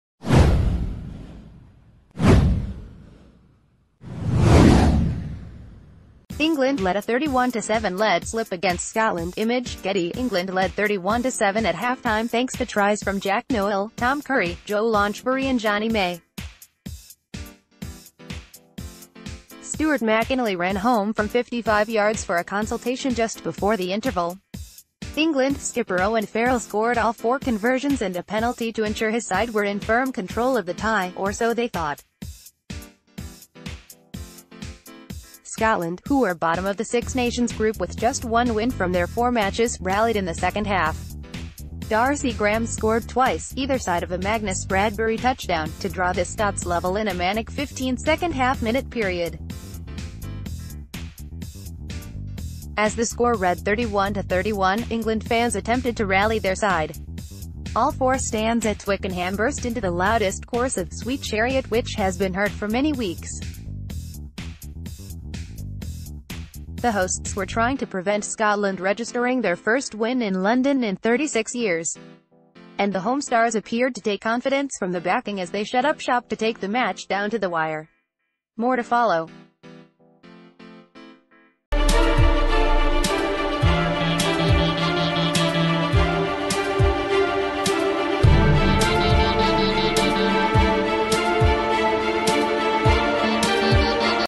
Six Nations: How England fans tried to stop Scotland collapse at Twickenham
England let a 31-7 lead slip against Scotland (Image: GETTY)England led 31-7 at half-time thanks to tries from Jack Nowell, Tom Curry, Joe Launchbury and Jonny May. Stuart McInally ran home from 55 yards for a consultation just before the interval. England skipper Owen Farrell scored all four conversions and a penalty to ensure his side were in firm control of the tie - or so they thought. Scotland, who are bottom of the Six Nations group with just one win from their four matches, rallied in the...